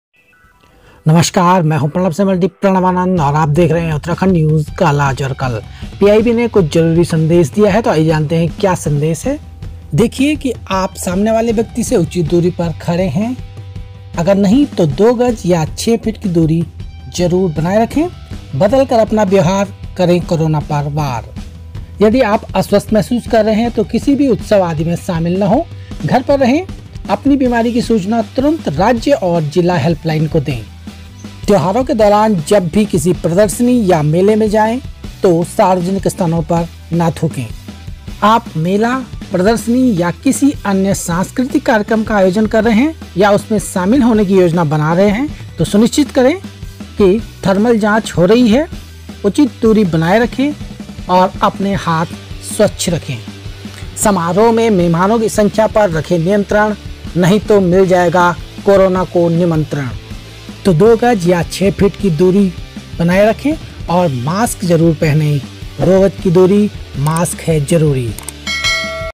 Tyoharon ke liye jaruri disha nirdesh l
Dosto es video mai ham tyoharon aur anya sarvjanik karyakarmo mai corona se bachav ki jankari lenge jo ki PIB ke dwara jari ki gayi hai.